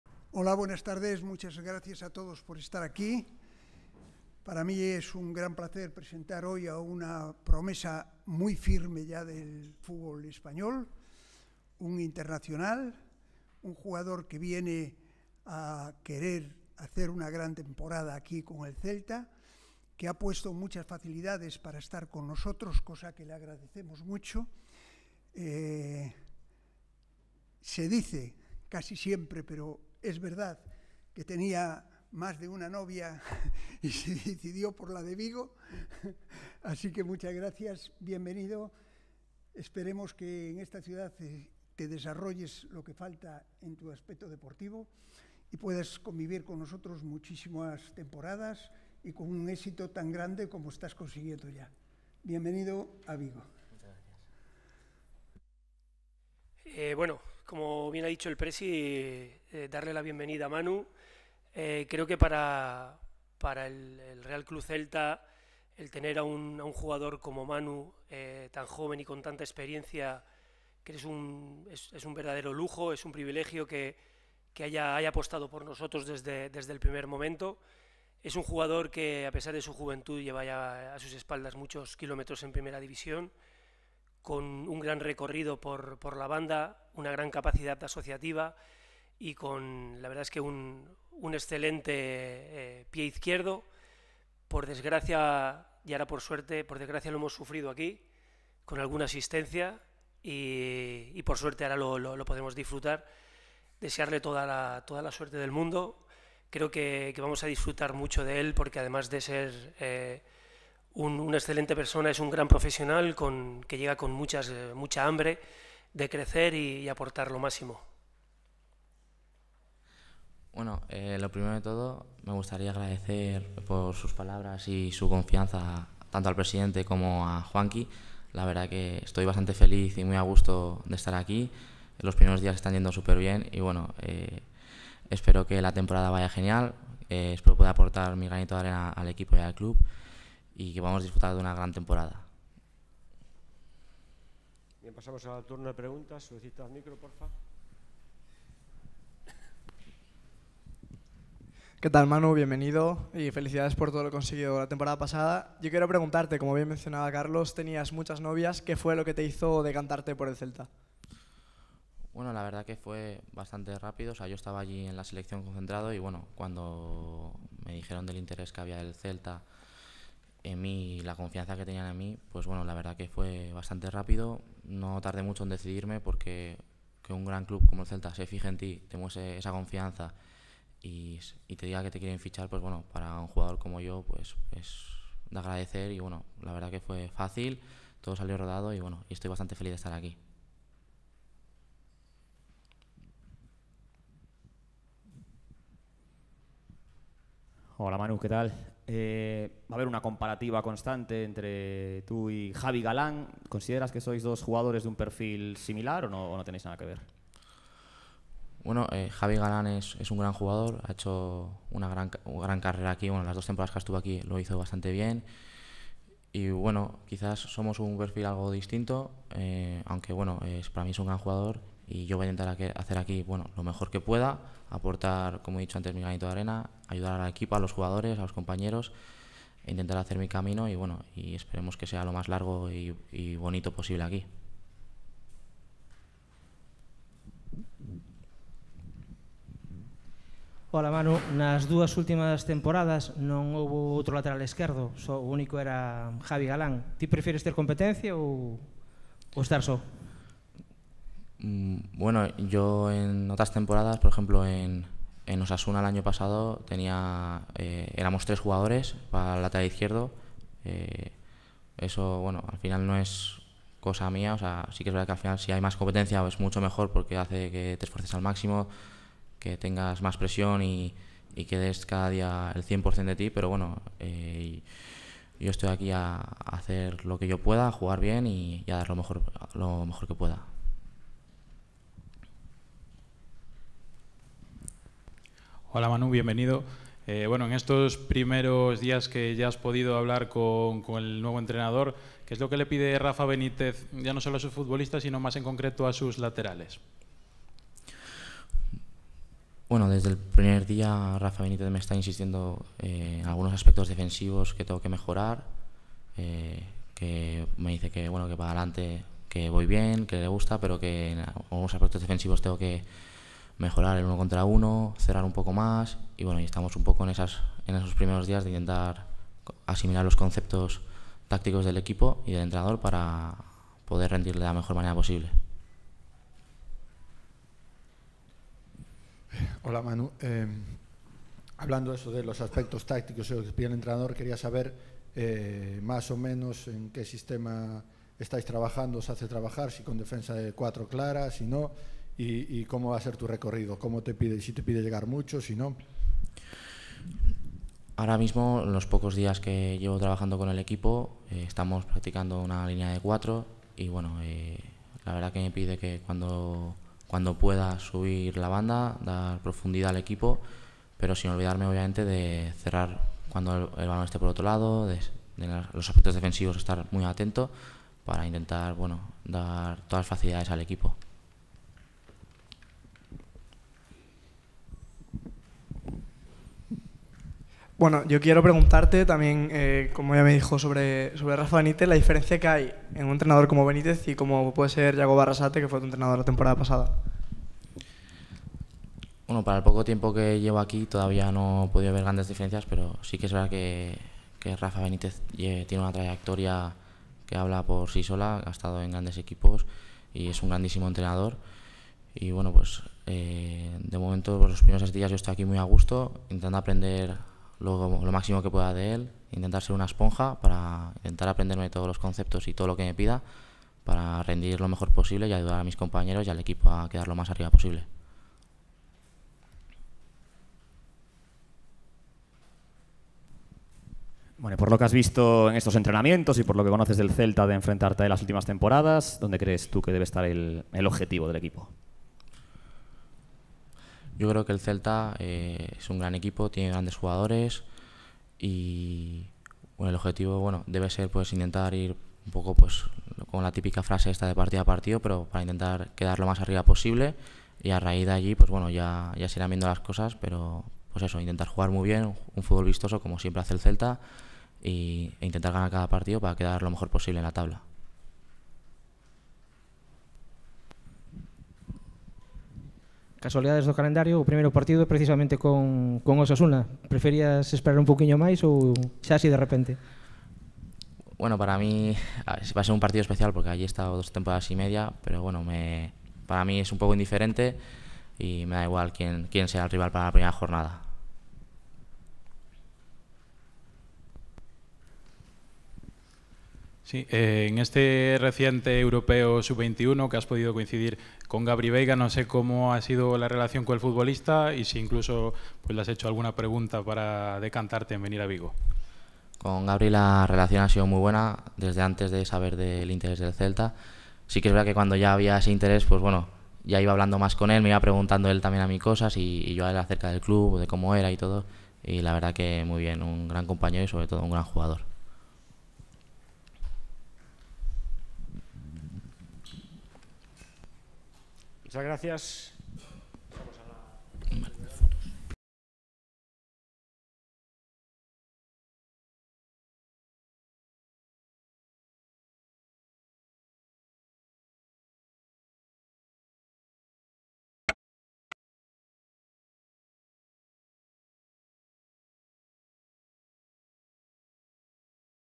Presentación oficial de Manu Sánchez como nuevo jugador del RC Celta 💙
Rueda de prensa de presentación oficial de Manu Sánchez, nuevo lateral izquierdo del RC Celta hasta 2028. Con apenas 22 años, internacional sub-21 con España y con destacadas actuaciones en CA Osasuna, ¡Manu ya luce la celeste!